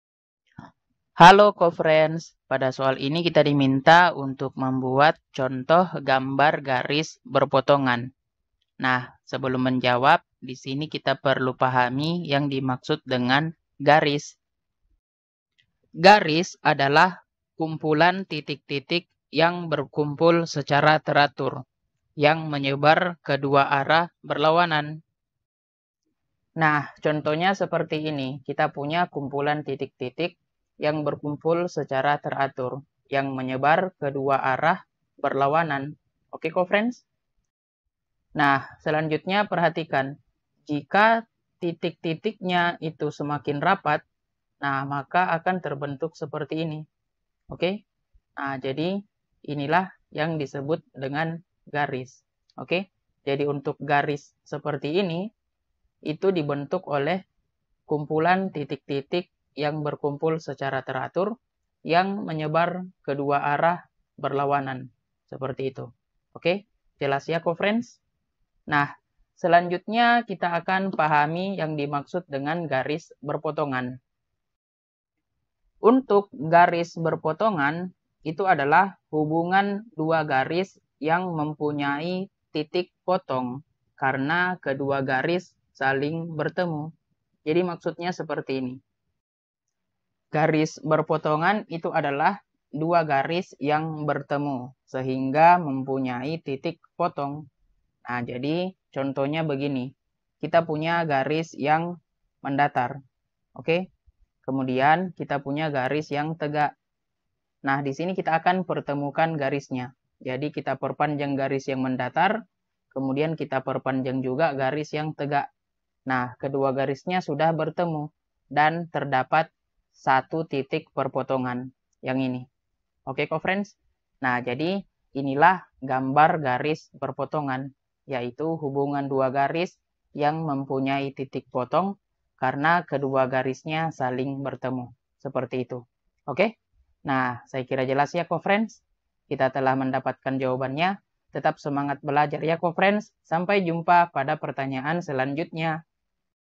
Buatlah contoh gambar garis berpotongan?
Topik atau Materi: Penyelesaian Masalah Bangun Datar (soal cerita atau gabungan bangun datar) - Bangun Datar - Geometri - Matematika Kelas 4 SD - K-13 Revisi

Sumber Soal: LKS Alif Matematika Kelas 4 Smt.2, K-13 Revisi, TH , N/A

00:00 : Bedah Soal
01:00 : Konsep, rumus dan pengertian Penyelesaian Masalah Bangun Datar (soal cerita atau gabungan bangun datar)
01:18 : Langkah penyelesaian soal
01:26 : Jawaban akhir
01:56 : Penutup